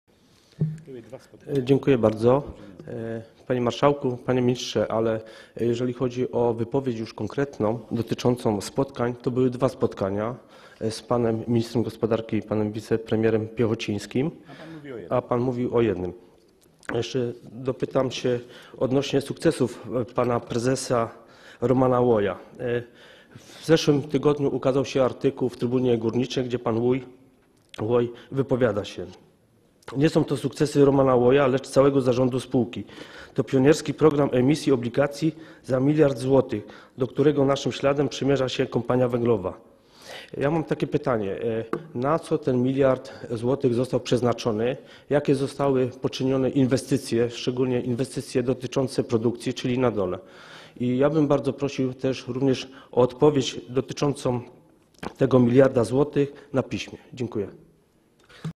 [17/316] Grzegorz Matusiak: Dziękuję bardzo. Panie Marszałku! Panie Ministrze! Jeżeli chodzi j..
Posiedzenie Sejmu nr 37
4 kwietnia 2013 r. 09:24:26
Poseł Grzegorz Matusiak:

Dziękuję bardzo.

Panie Marszałku! Panie Ministrze! Jeżeli chodzi już o konkretną wypowiedź dotyczącą spotkań, to były dwa spotkania z panem ministrem gospodarki, panem wicepremierem Piechocińskim, a pan mówił o jednym.

Chciałbym jeszcze dopytać o sukcesy pana prezesa Romana Łoja. W zeszłym tygodniu ukazał się artykuł w „Trybunie Górniczej", gdzie wypowiedział się pan Łój: Nie są to sukcesy Romana Łoja, lecz całego zarządu spółki. To pionierski program emisji obligacji za miliard złotych, do którego naszym śladem przymierza się Kompania Węglowa.

Mam pytania. Na co został przeznaczony ten miliard złotych? Jakie zostały poczynione inwestycje, szczególnie te dotyczące produkcji, czyli na dole? Bardzo proszę o odpowiedź dotyczącą tego miliarda złotych również na piśmie. Dziękuję.